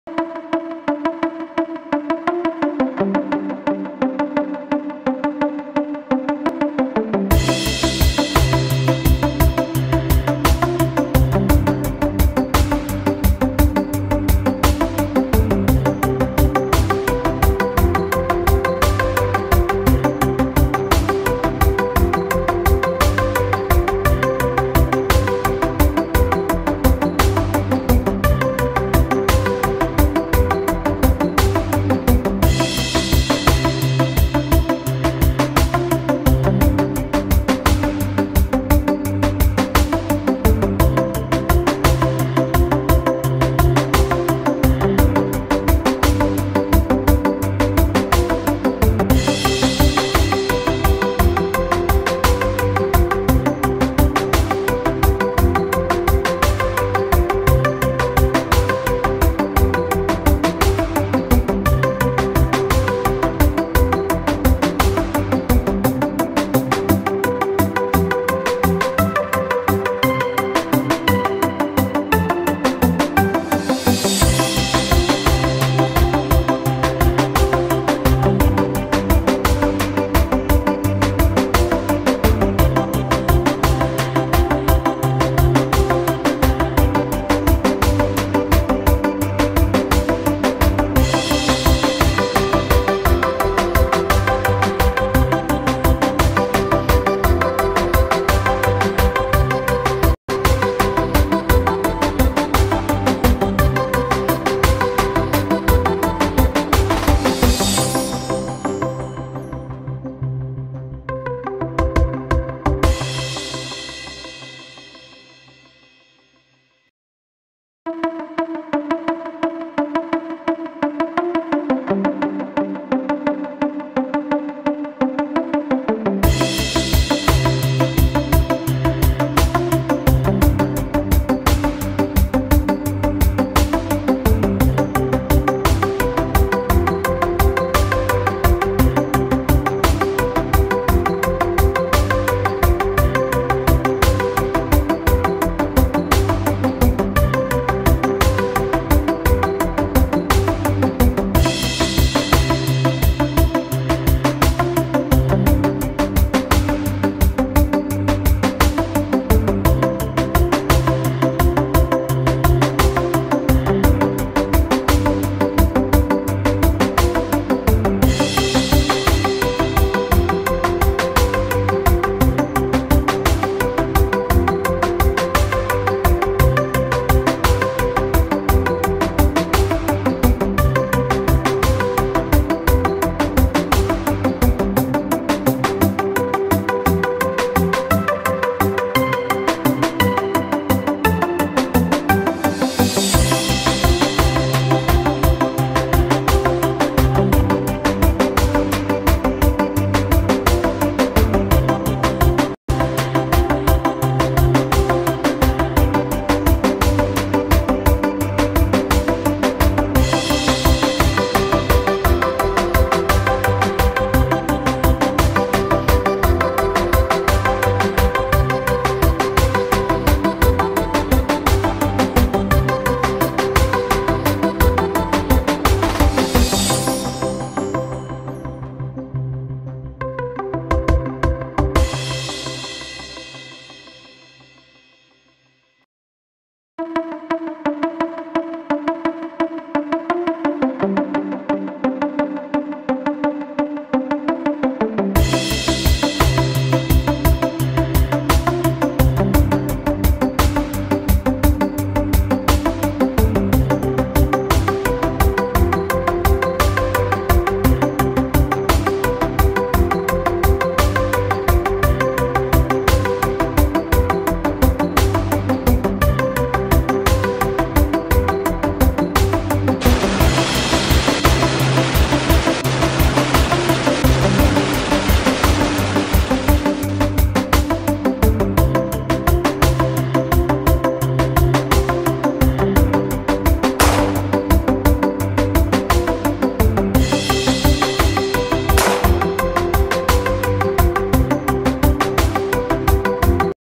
PYRAMID PERFORMANCE by TWELFTH BOYS of SRI VIDHYA BHARATI MHSS, SALEM 10 on TEACHERS' DAY 5/09/19
PYRAMID PERFORMANCE Fabulous Pyramid Performance by Twelfth Boys of Sri VIDHYA BHARATI MHSS, Neikarapatti, Salem 10 in Teachers' Day Celebration on 5th September 2019